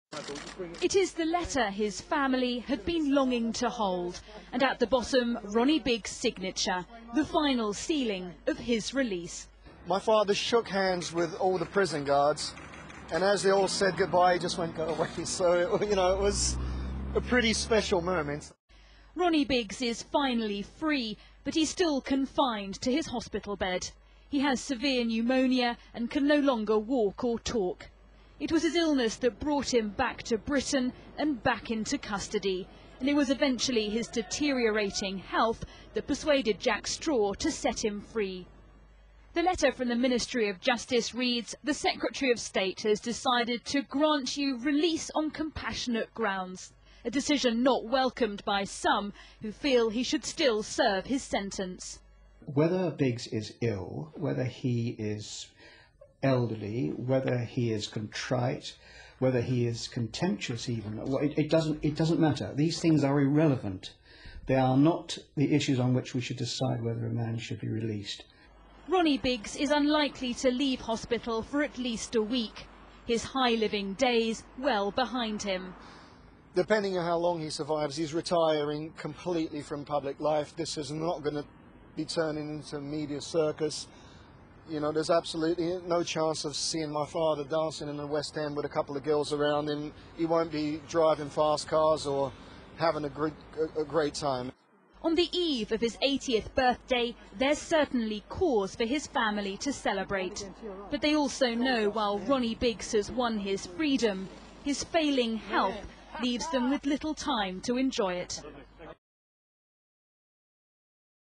Ronnie Biggs:Great Train Robber Released After Years In Jail
Great Train Robber Ronnie Biggs is a free man after being officially released from his prison sentence on compassionate grounds. He's currently seriously ill.